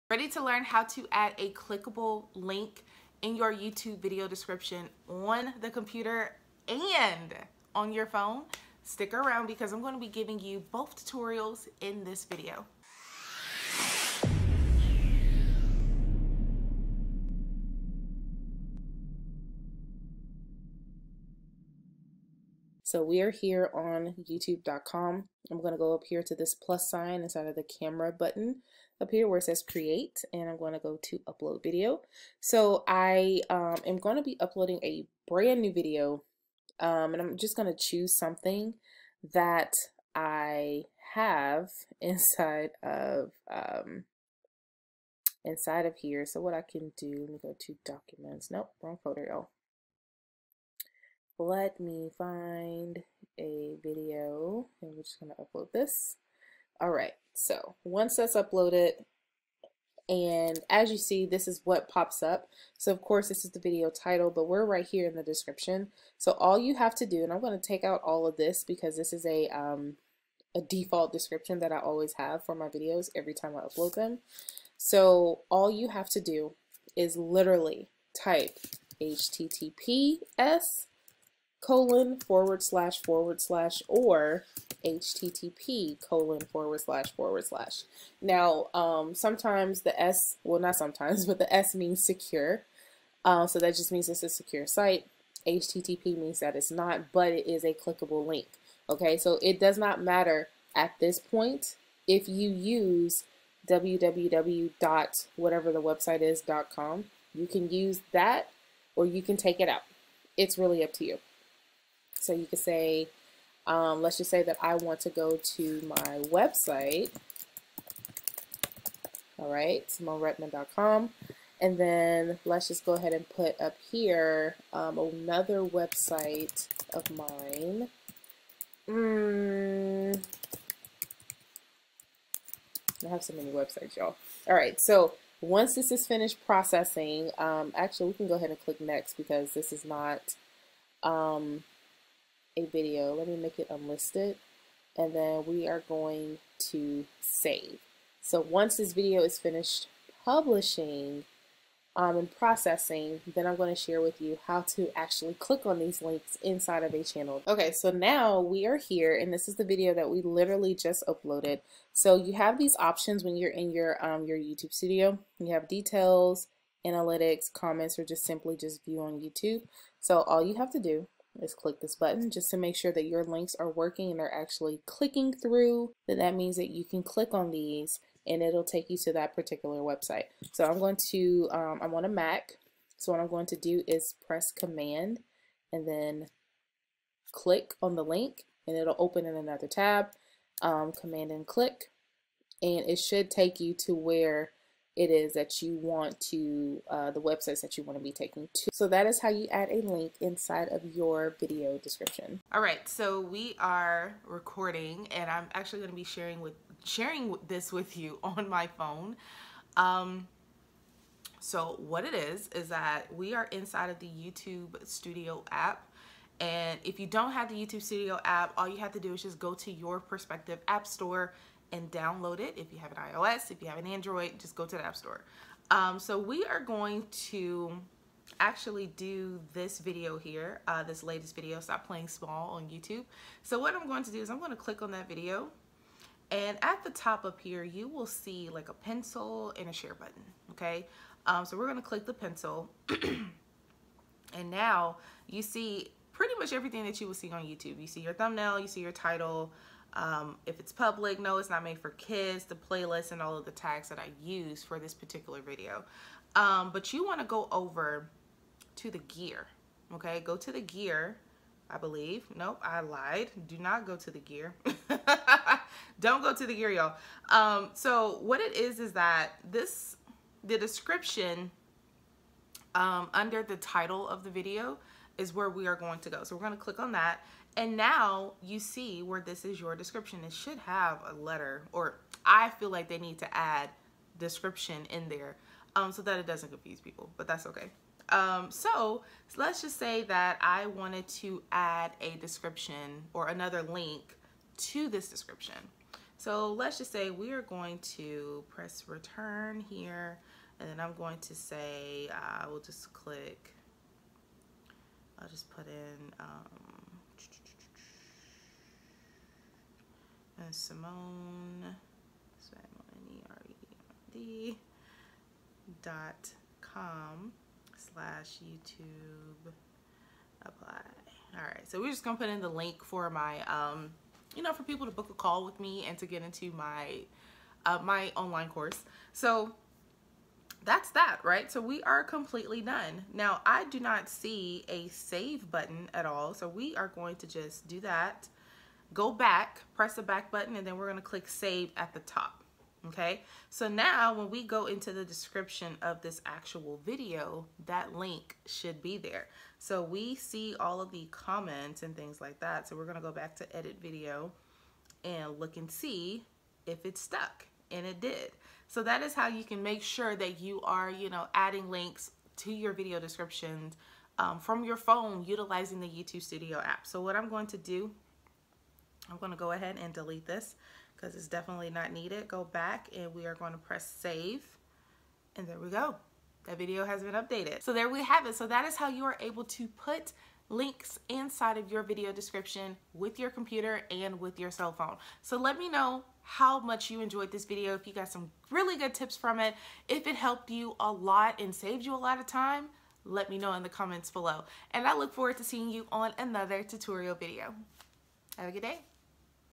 How To Insert Links In YouTube Description - PC and Cell Phone Tutorial
Hey there! In this video, you will learn how to add clickable links in your YouTube description using your computer and on your cell phone.

Being able to add links into the description of your videos is a great way to drive traffic to your websites if you haven't achieved monetization status on YouTube yet.

I've always used clickable links in my video descriptions and it helps to bring in more leads and sales for my online business.

Sharing YouTube and business tips to help you grow your YouTube channel and your online business faster than you thought possible!